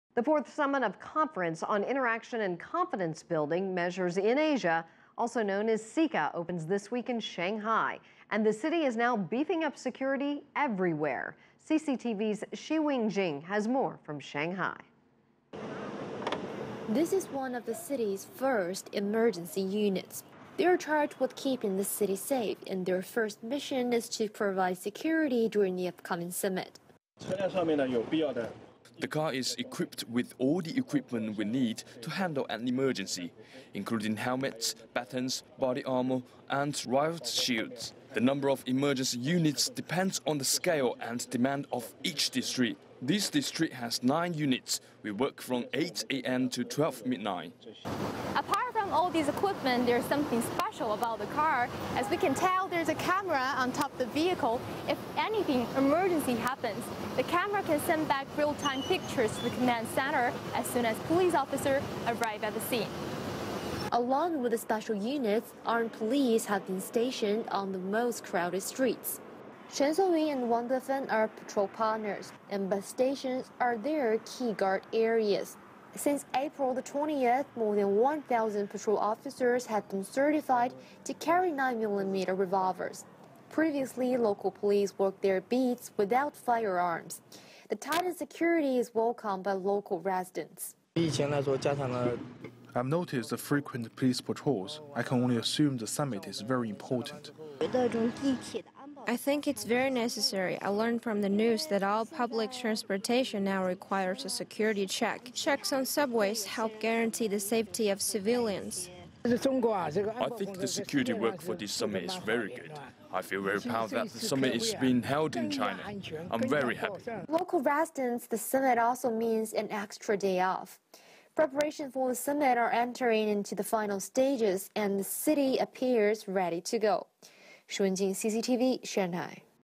China Sets Tightened Security for CICA
Shanghai is beefing up security ahead of the CICA summit from May 20th to 21st. CCTV's Shi Wenjing visited one of the commercial centers.